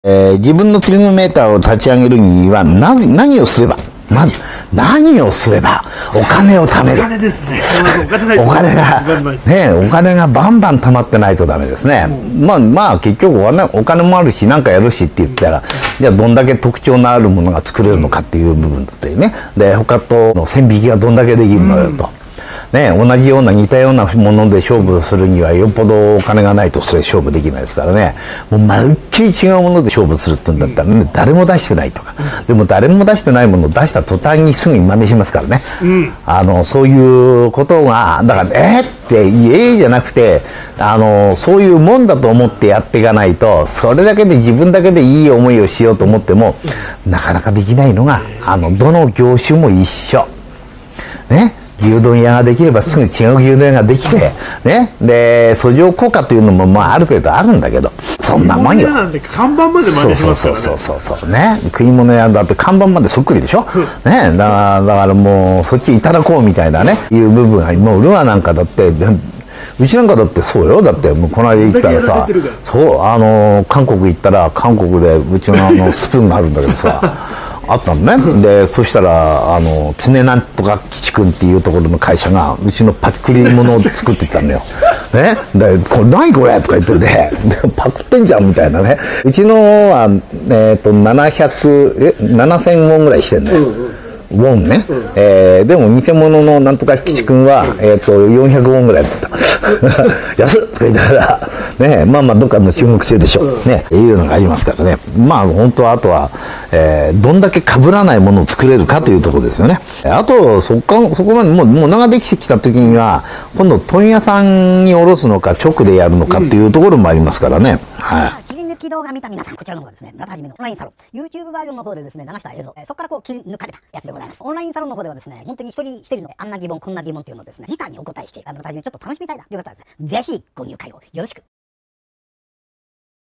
【村田基】このメーカーにウチのルアーをパクられました【村田基切り抜き】
あるメーカーにウォーターランドのルアーをパクられたそうです。一体どこのメーカーでしょうか？
今回は村田さんがそんな疑問にお答えしています。

　　生配信先！
〇潮来釣り具センター
　ジムのお店！